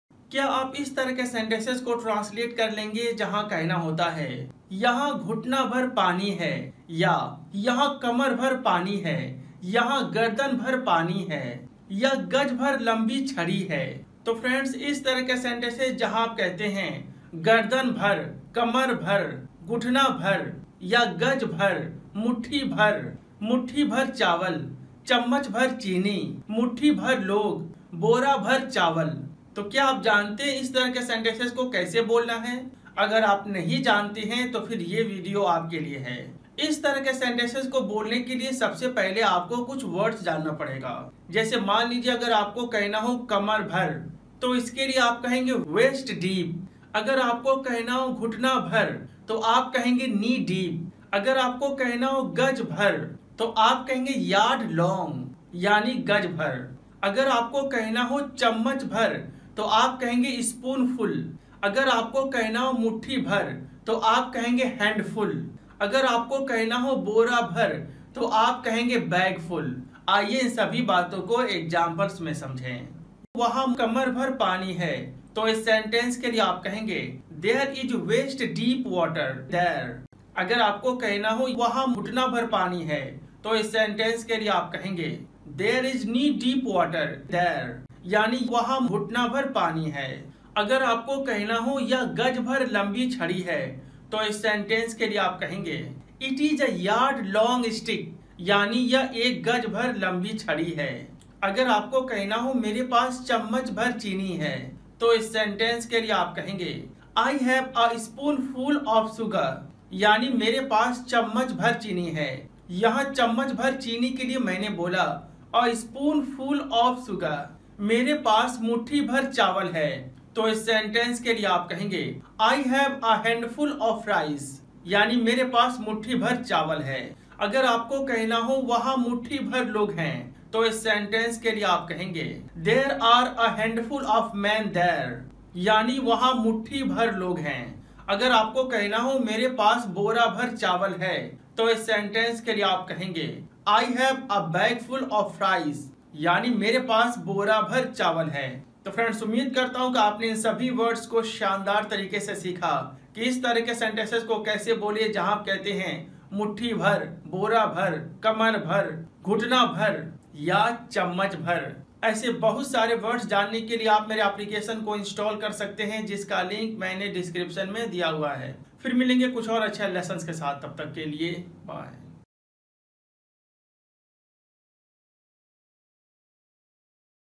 Daily used English sentences | अंग्रेजी बोलना कैसे सीखे | English speaking Practice sentences.
Website Linkl

कमर-भर  - Waist-deep
घुटना-भर  - knee-deep
गज़-भर  -  yard-long
चम्‍मच-भर - spoonful
मुट्ठी -भर - handful
बोरा-भर - bagful

Friends चलिए देखते है कि इन words
*कमर-भर  - Waist-deep
घुटना-भर  - knee-deep
गज़-भर  -  yard-long
चम्‍मच-भर - spoonful
मुट्ठी -भर - handful
बोरा-भर - bagful*
का use कैसे और कैसे करे

Here are some examples 👇

Waha kamar-bhar pani hai. There is waist-deep water there.

Waha ghutna-bhar pani hai
There is knee-deep water there.

Yah gaz-bhar lambi chhadi hai .
It is a yard-long stick.

Mere pass chammach-bhar chini hai.
I have a spoonful of suger.

Mere paas muthhi-bhar chawal hai.
I have a handful of rice.

Waha muthathi-bhar log hai. There are a handful of men there.

Mere paas bora-bhar chawal hai .
I have a bagful of rice.